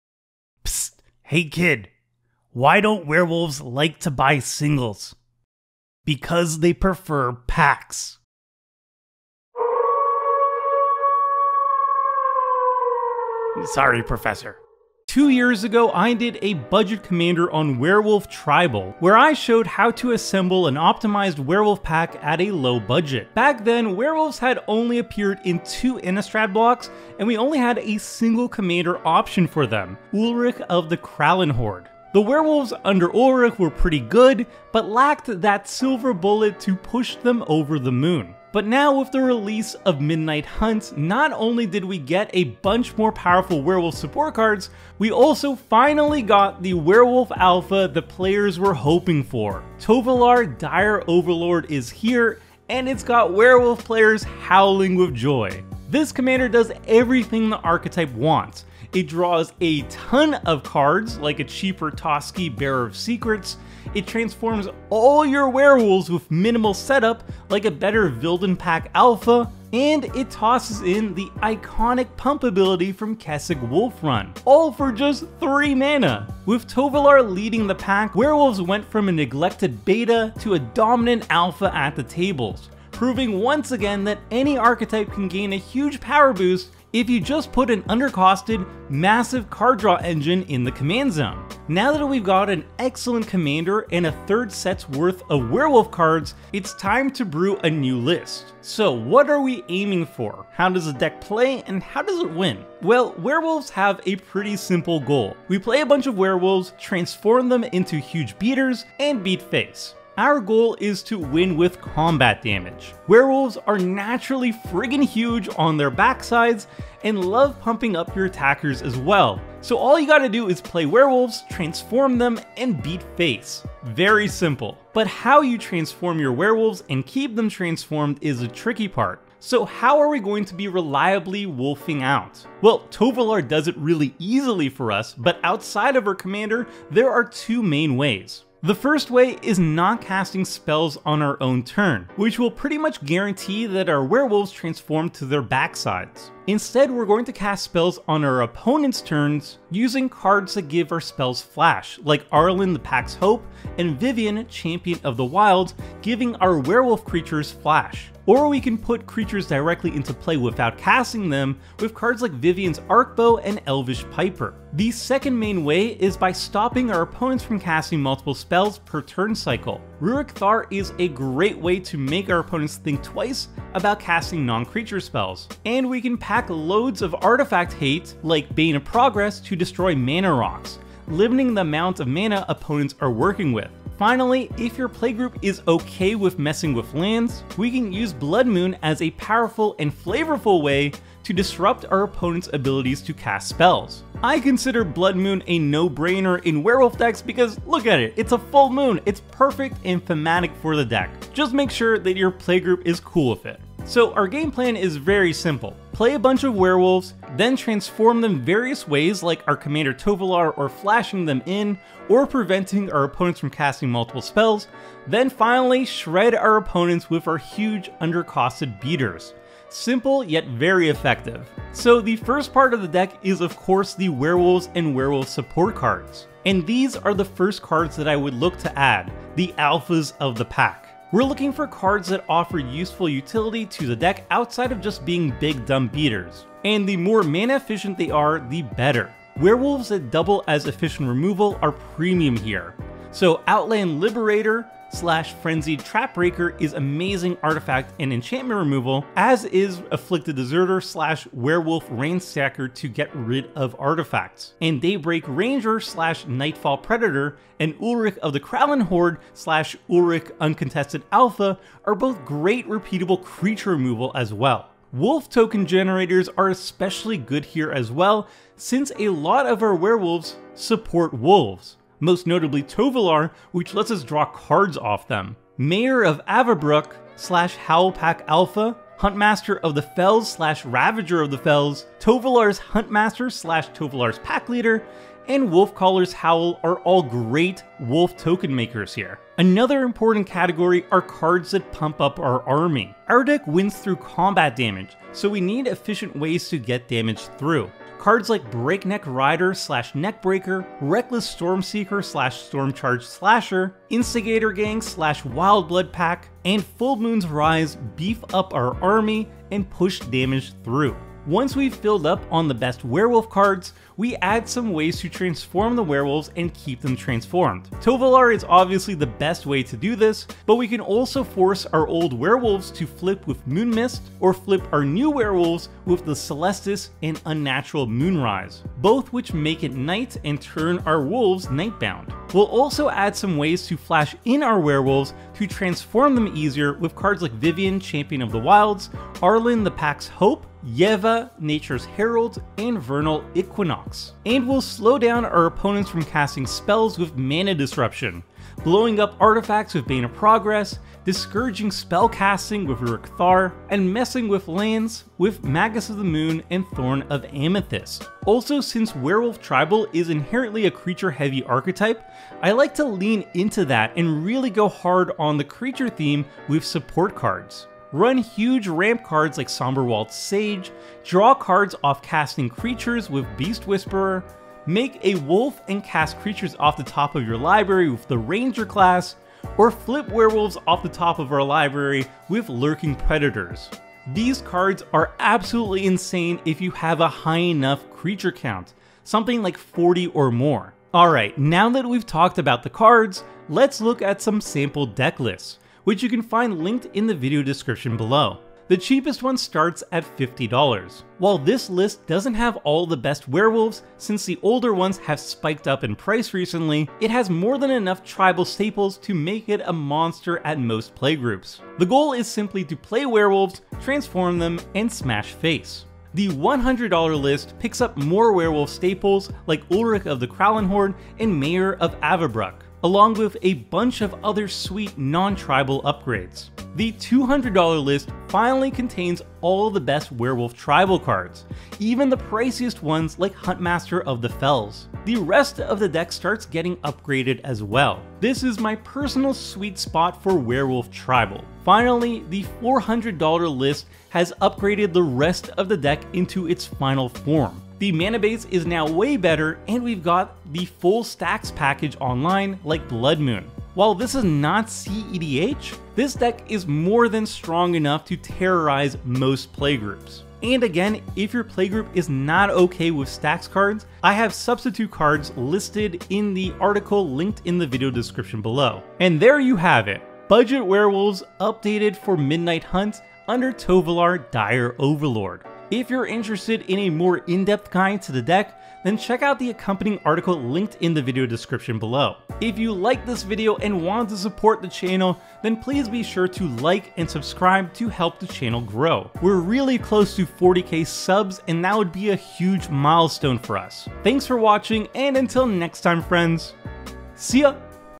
How To Build The Best Werewolf Deck on a Budget | Tovolar | $50 | Budget Commander MTG
0:00 Intro
00:50 Our Commander
01:38 Deck Game Plan
03:42 Our Best Cards
06:30 Deck Lists